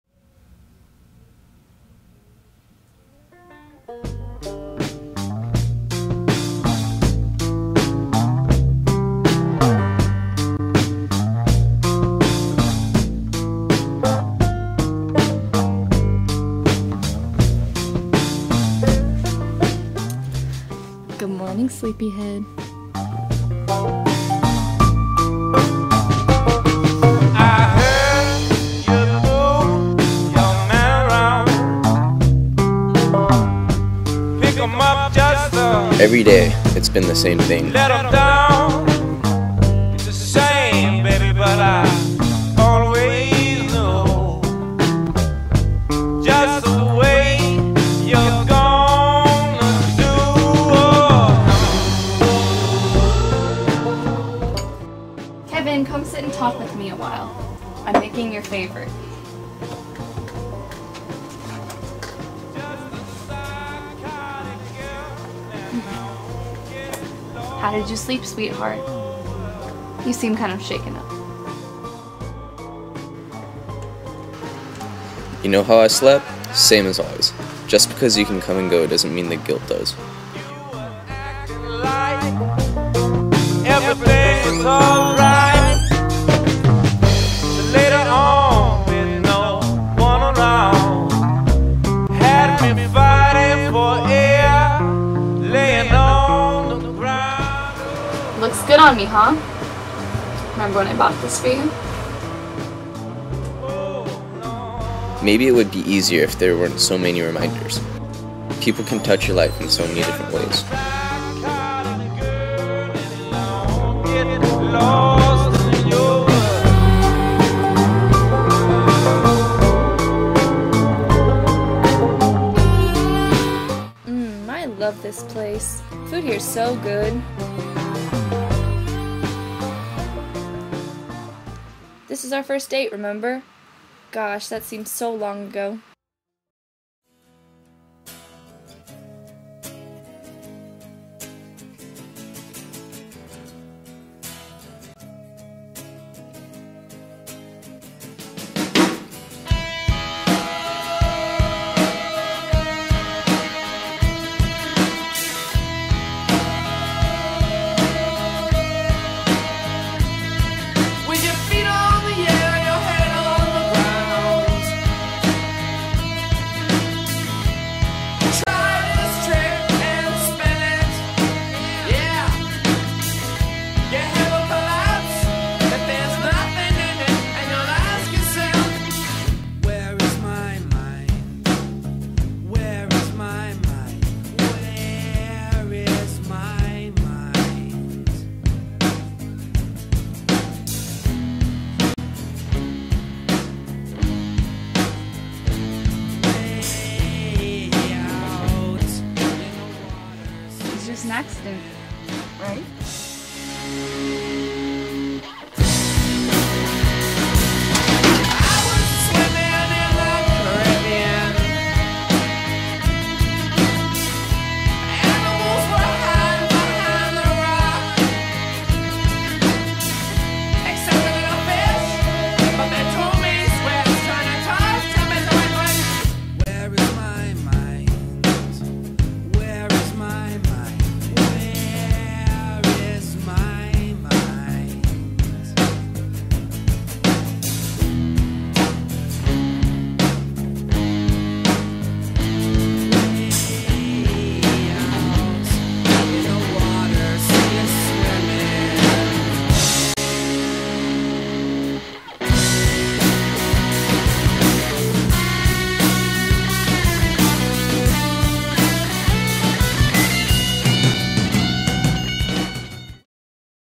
Kevin and Sasha
Short film project for High Tech High North County's multimedia class.